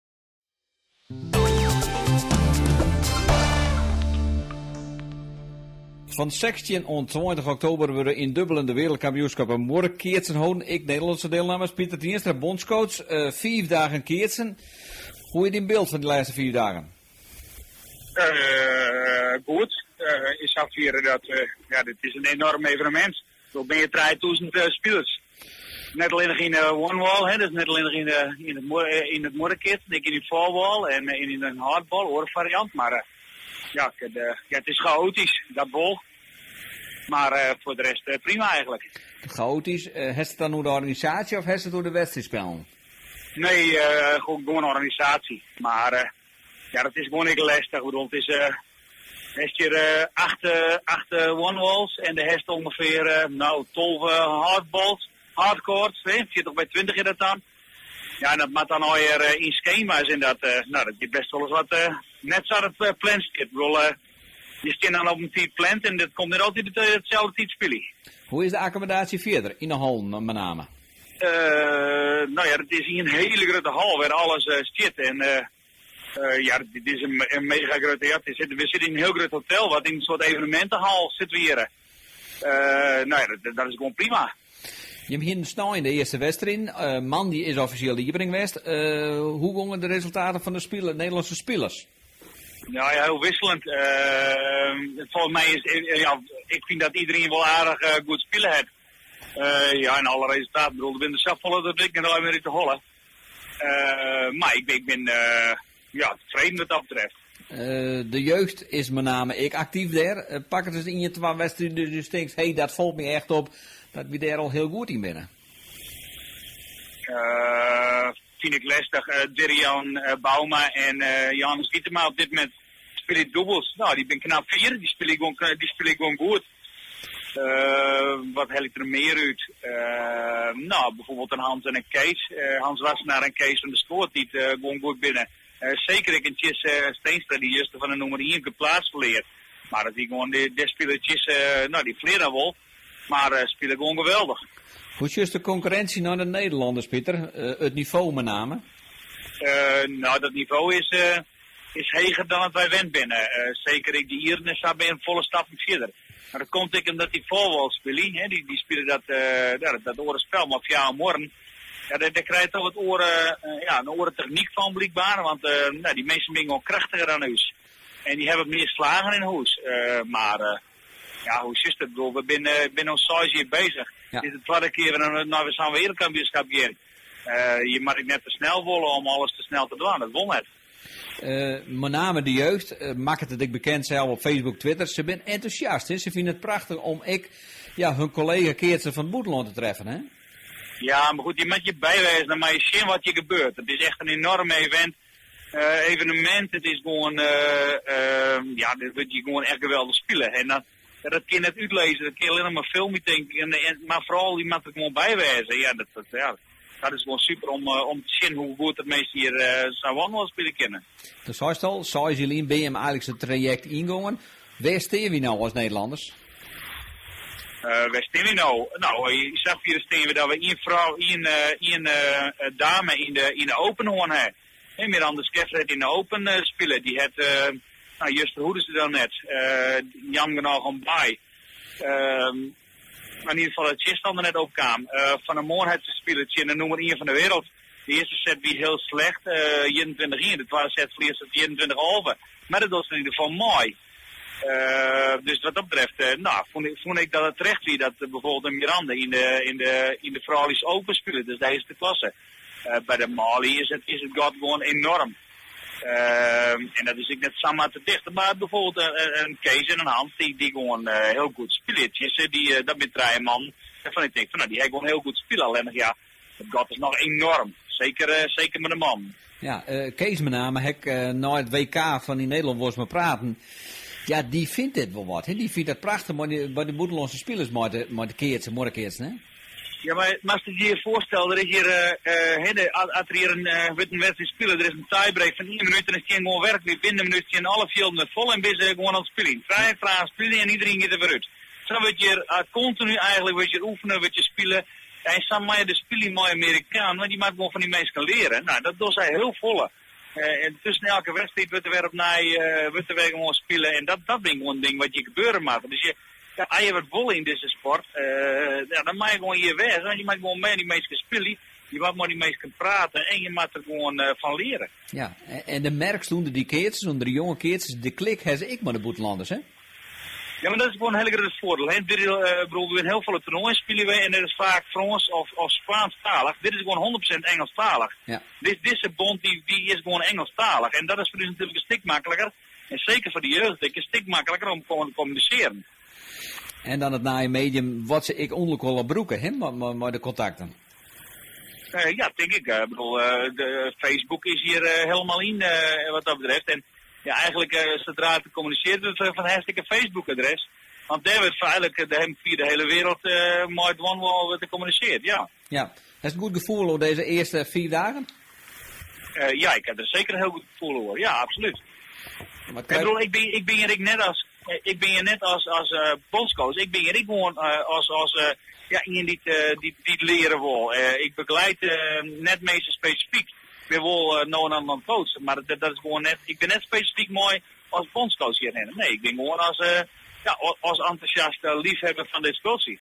Pieter Tienstra tevreden over kaatsers in Dublin
Van 16 - 20 oktober is het WK murkaatsen in Dublin. Een Friese delegatie jeugd en senioren kaatsers slaan voor best op.